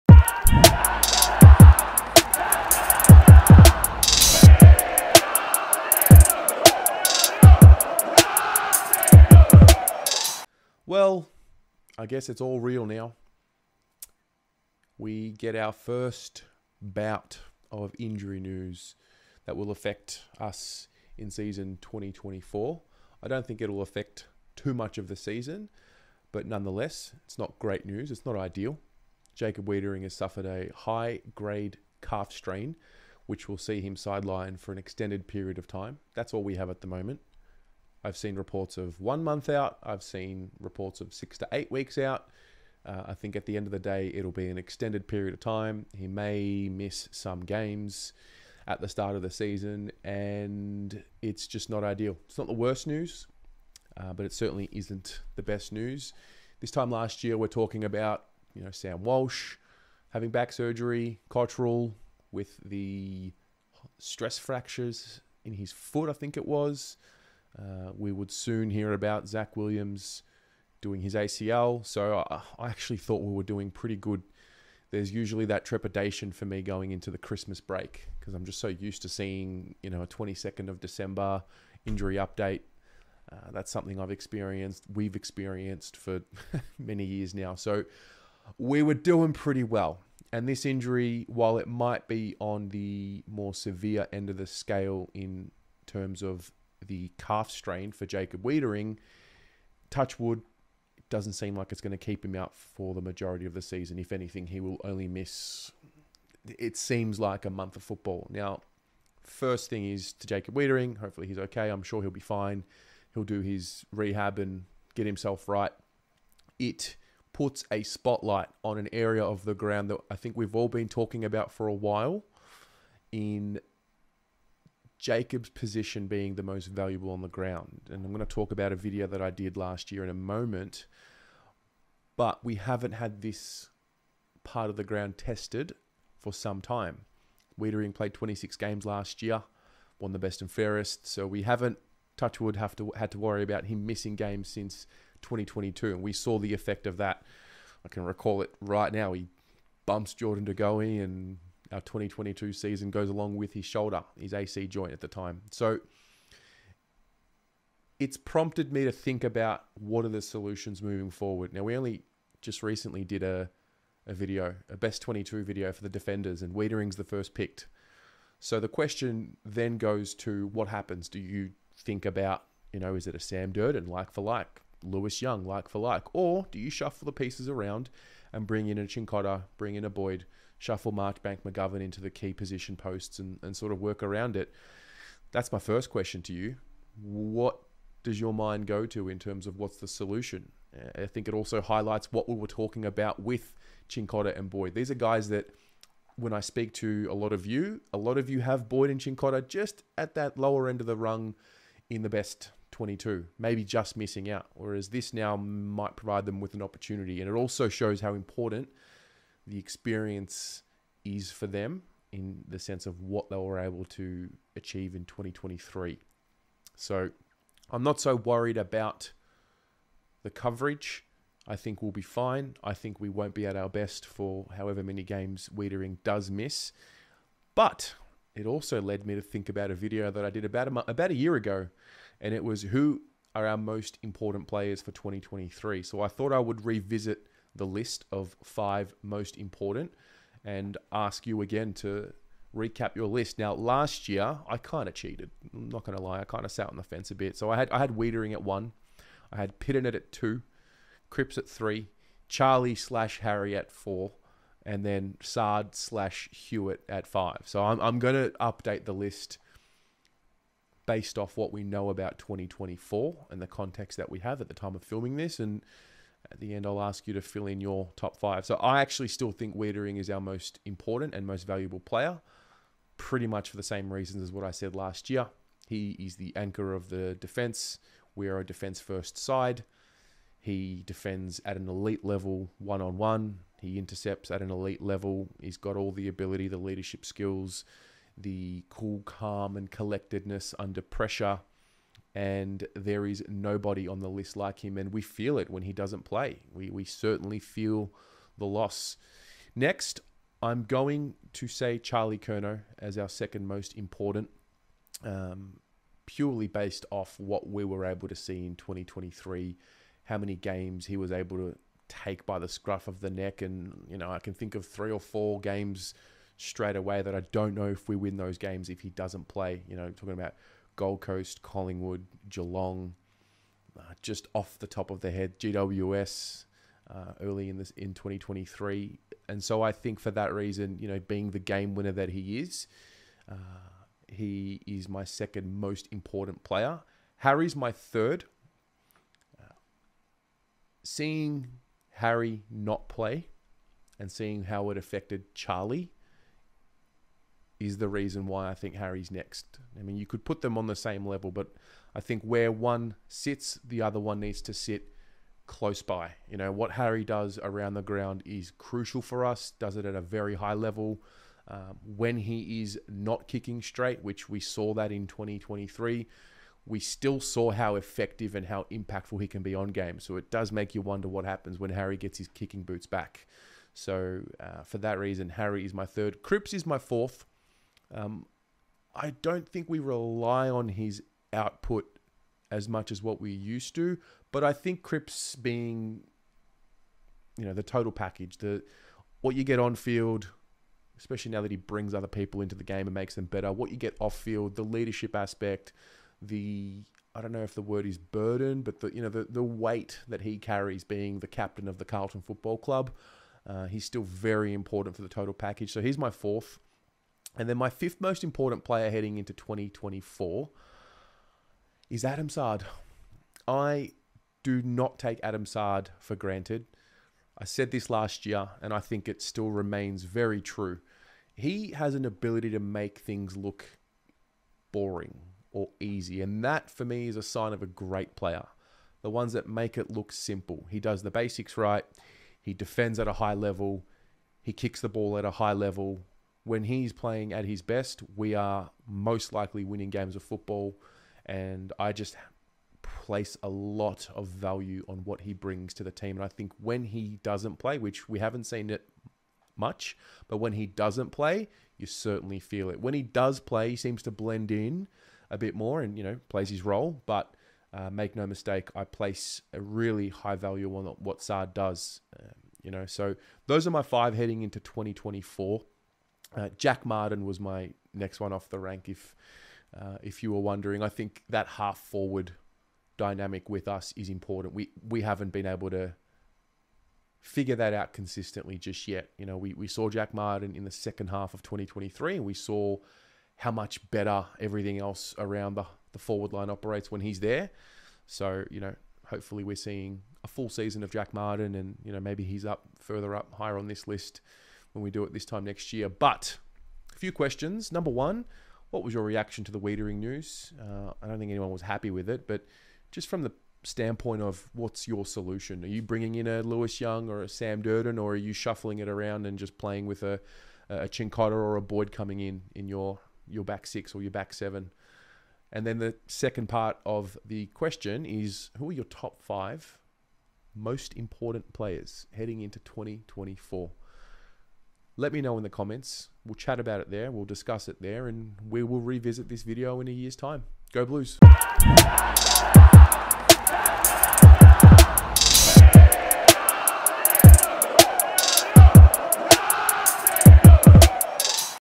Jacob Weitering Injury Update | Who is Carlton's Most Important Player?
🔗 Description: An update on the hamstring injury suffered by Jacob Weitering and a look at who our most important players are.

🔵 Blue Abroad: The Carlton Fan Channel